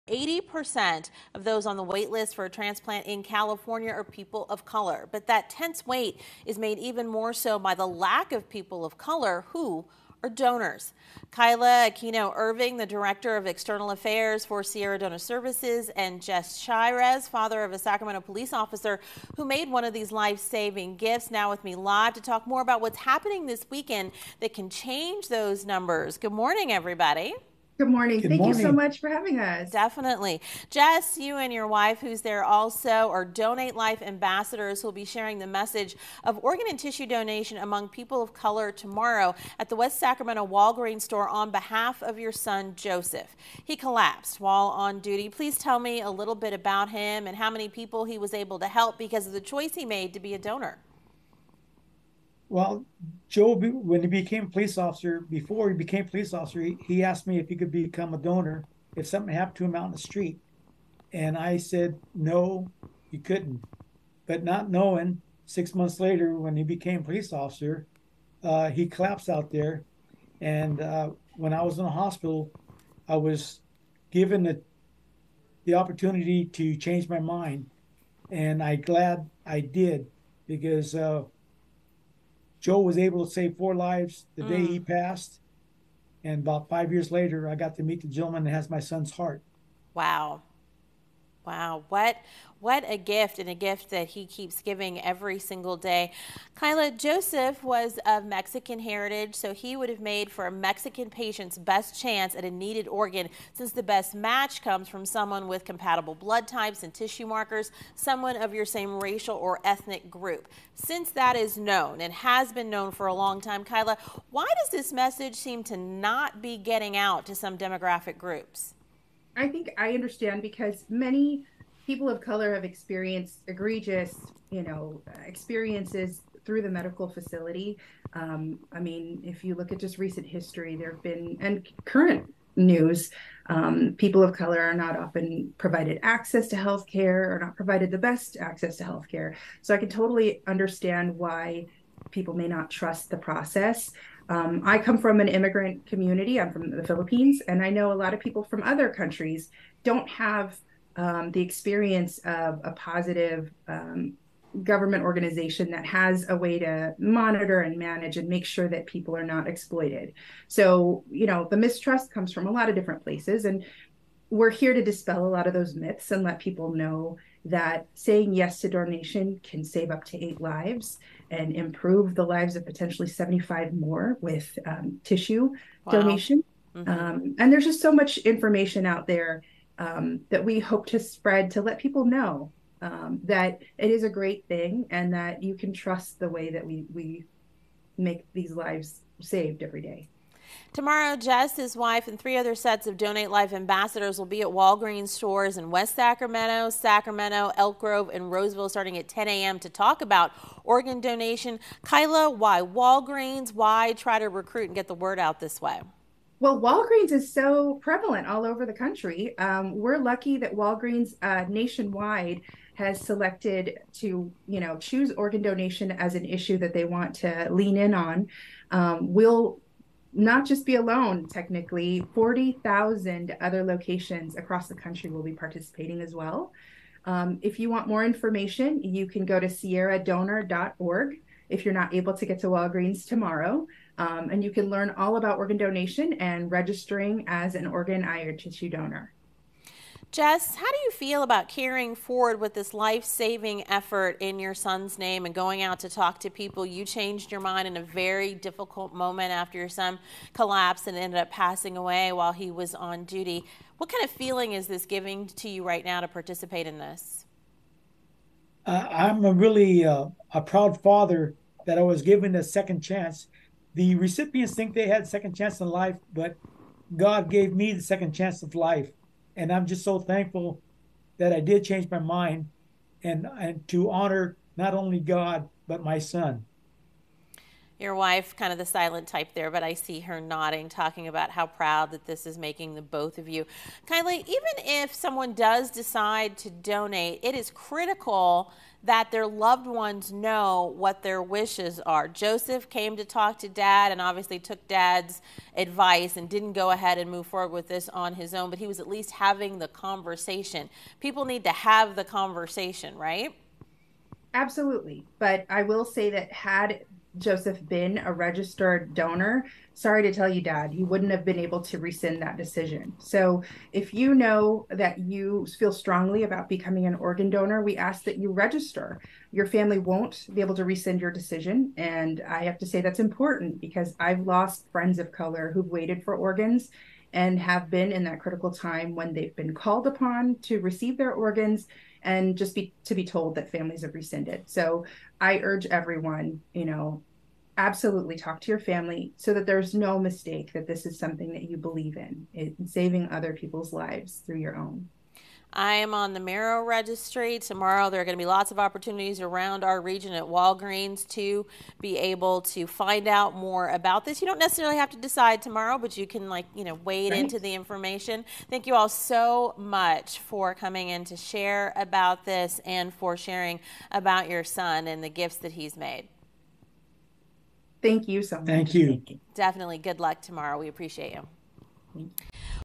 Organ Donation Lacking for People of Color
Jess Chairez, the father of a Sacramento organ donor and Sierra Donor Services External Affairs Director Kyla Aquino Irving discusses the difficulties that people of color face when needing an organ transplant and the gifts that Chairez's son gave when he saved five people's lives with his donated organs.